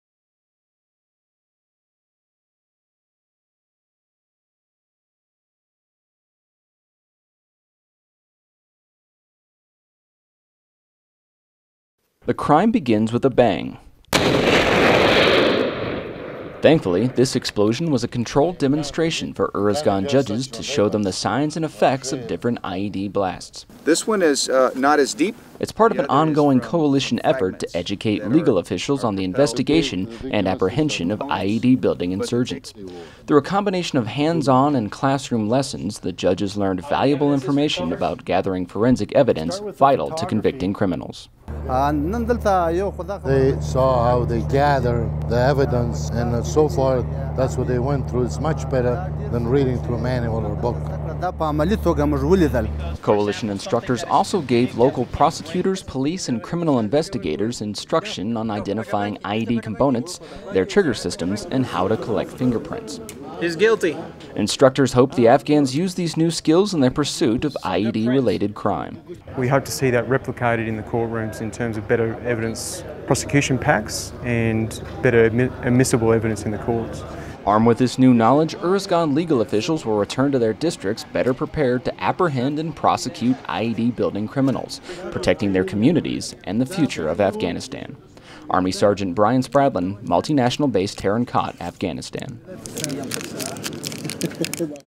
Afghan Criminal Investigation Training - 16th MPAD - SGT Bryan Spradlin
Improvised explosive devices are a major threat to the people of Uruzgan province ad it's up to the Afghan legal system to bring the criminals who build and plant them to justice. SGT Bryan Spradlin shows how Coalition Forces are equipping local legal officials with the skills they need to put the bad guys behind bars.